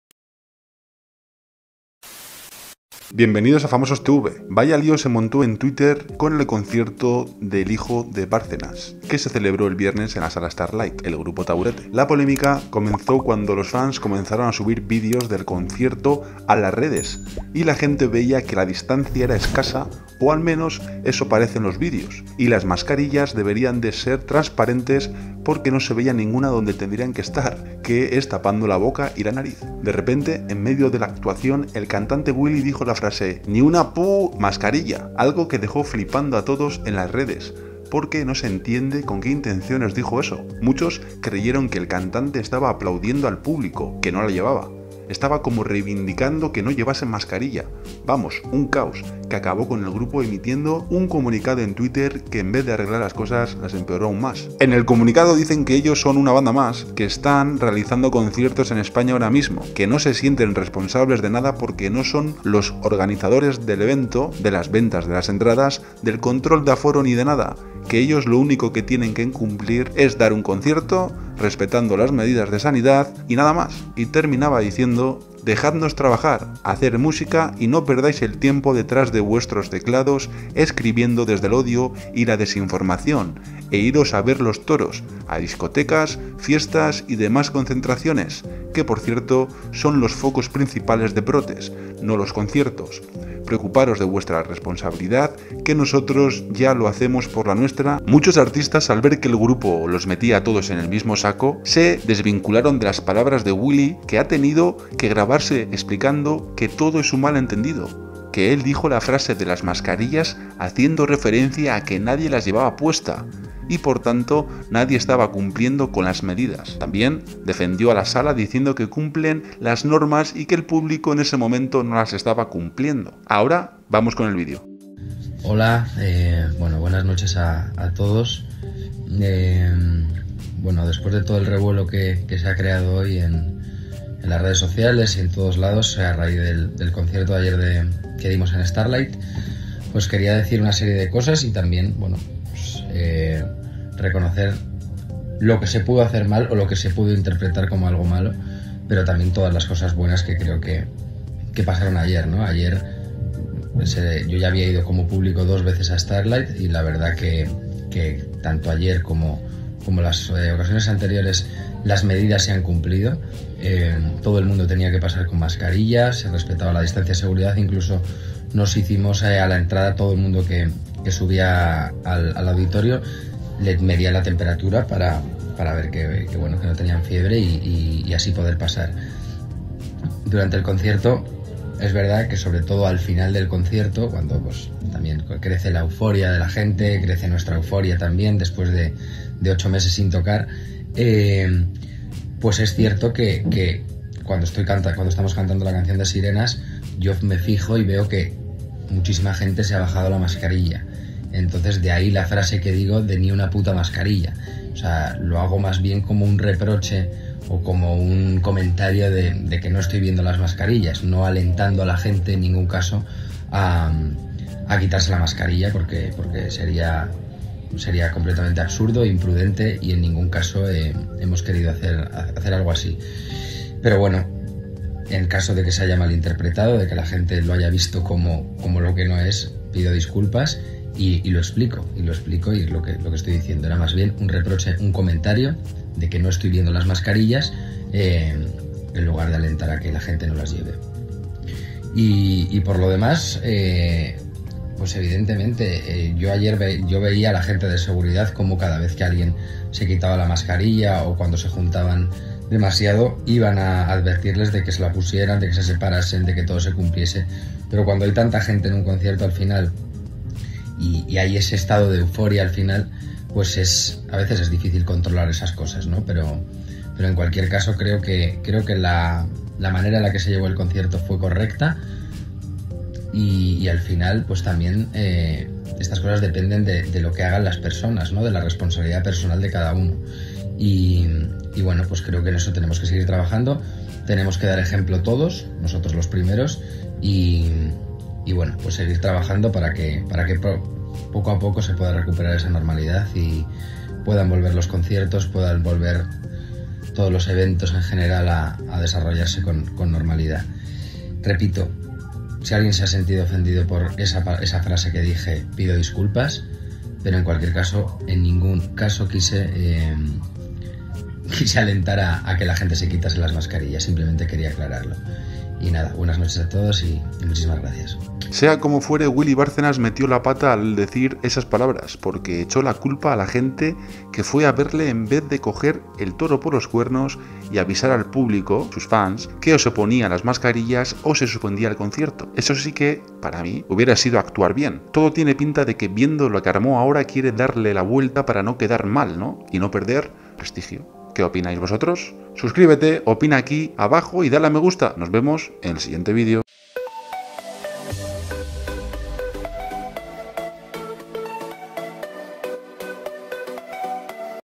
WILLY BARCENAS CONFIESA lo que PASO en el CONCIERTO STARLITE: ''Ni una P* mascarilla''
Taburete, grupo liderado por Willy Bárcenas ha estado en el punto de mira por celebrar un concierto sin mucho control sanitario y por unas declaraciones que nadie entendió.